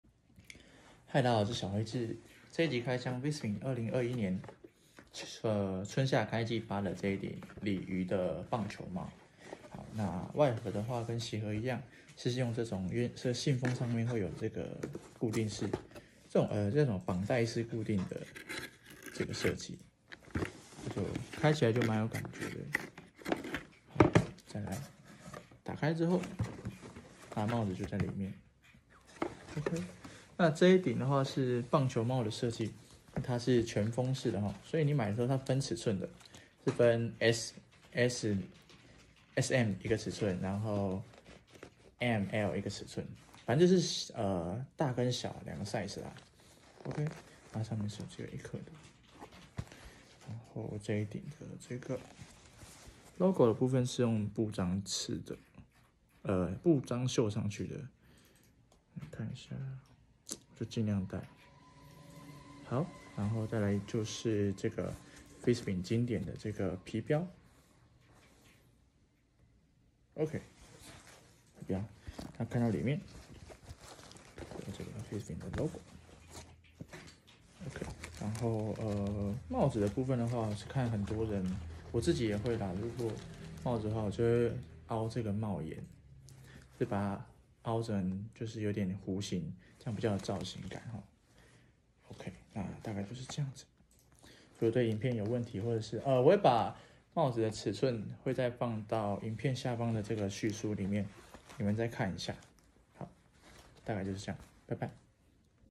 小黑痣【開箱】VISVIM 2021SS EXCELSIOR CAP 羊毛 鯉魚繡章 棒球帽
EXCELSIOR CAP
COLOR : BLACK , GREY , CAMO PATTERN
SIZE : S/M , M/L

Gabardine baseball cap made from wool-linen blend fabric
Original patch detail

Please Note :
83% Wool , 17% Linen
S/M : Circumference 22.8 (inch)
M/L : Circumference 23.6 (inch)
Please be aware that the fabric used may vary in color and pattern from item to item.

作為代購從業者，想多分享一些實品給大家，
目前就是用手機簡單拍攝與介紹。

更多訊息，或者聯繫我
你可以透過 小黑痣. 唯一官方Instagram